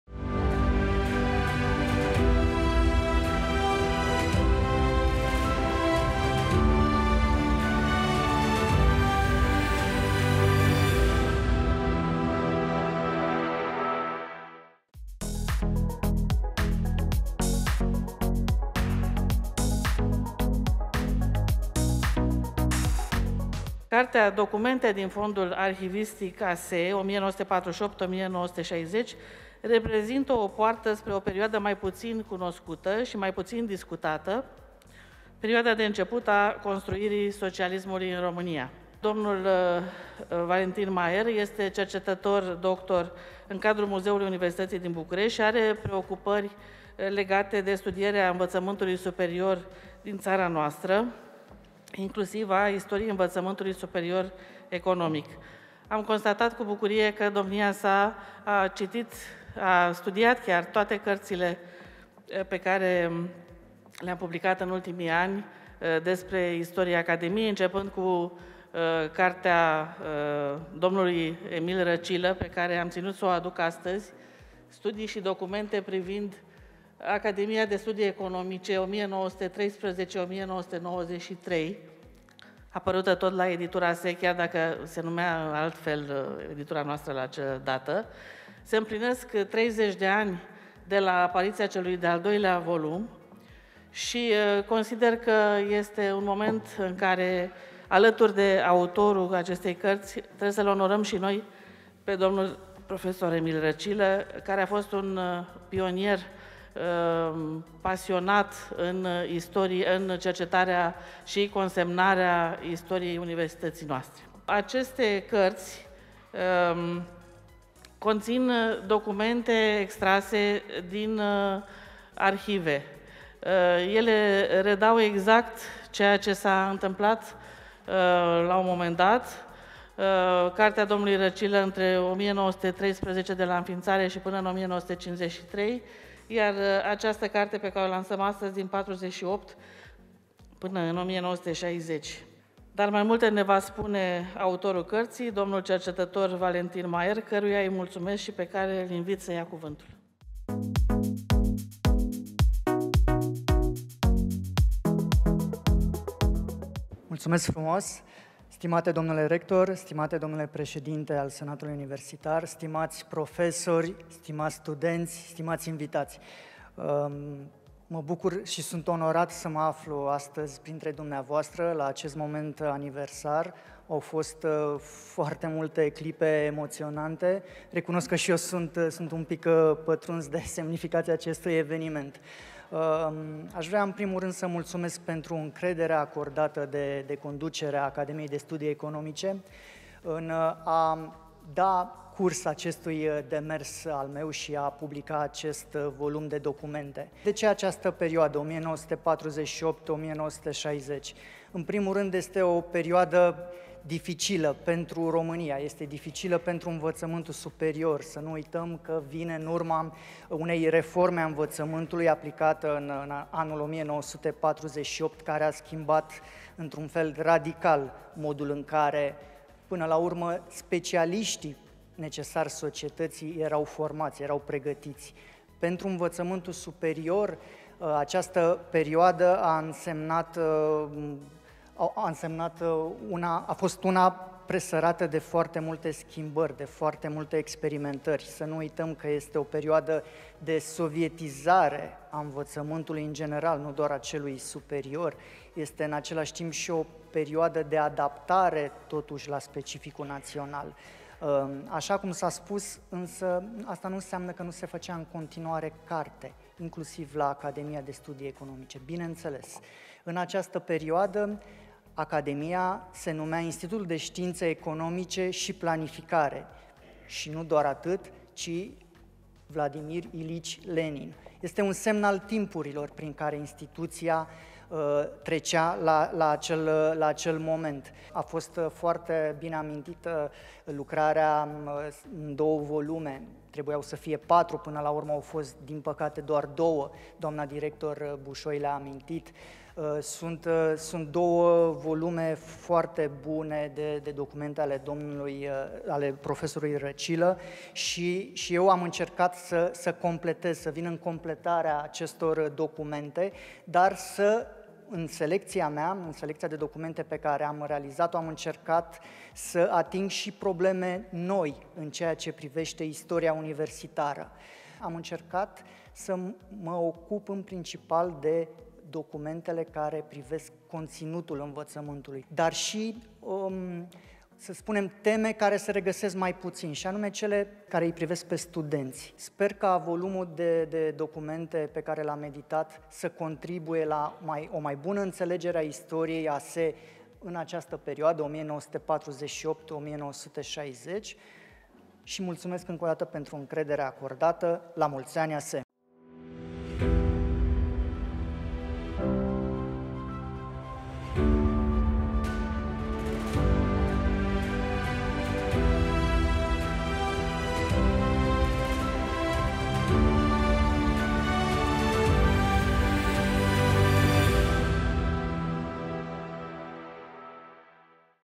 Lansare carte: „Documente din fondul arhivistic ASE, 1948-1960”, Valentin Maier, Editura ASE, 2024.
Adunarea festivă cu prilejul Zilei ASE - miercuri, 3 aprilie 2024